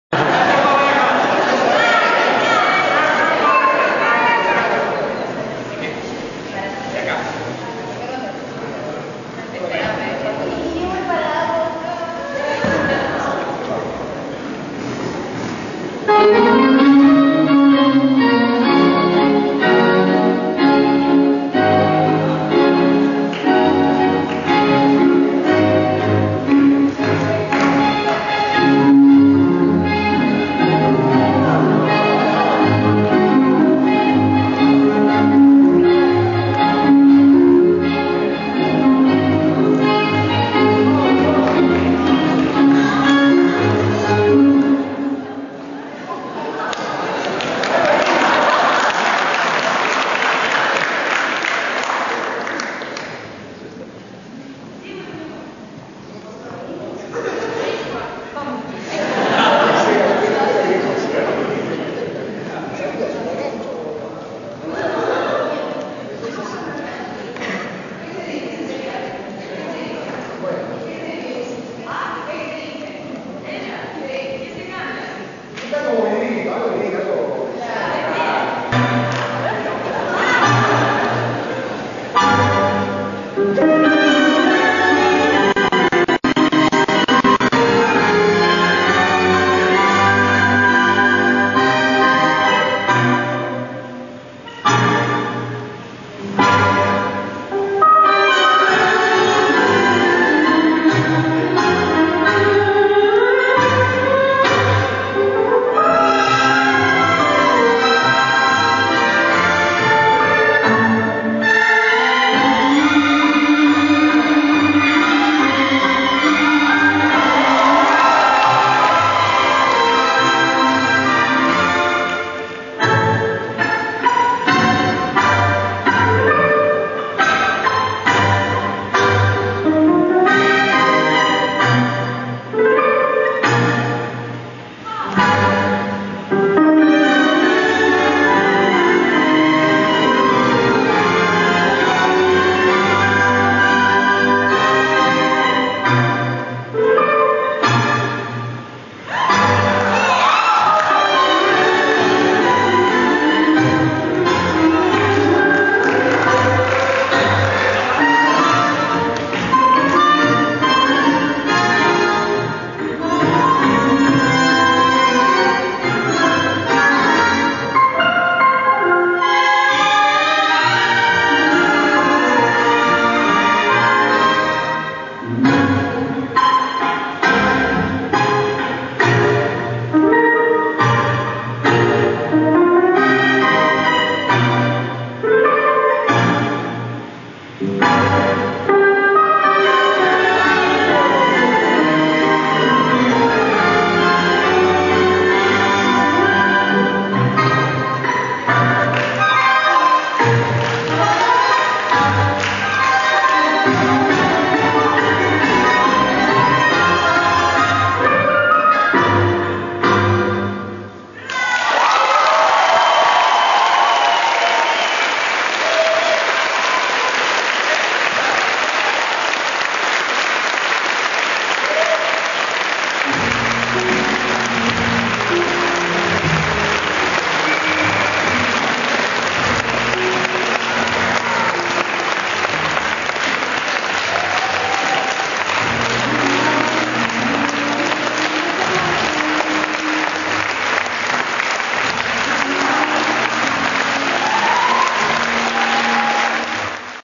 Stars together at WNT2008 (tango)
Marcela Guevara & Stefano Giudice, Silvina Valz & Oliver Kolker, Diego Riemer "El Pajaro" & Maria Belen Giachello  at White Nights Tango festival 2008, St.Petersburg, Russia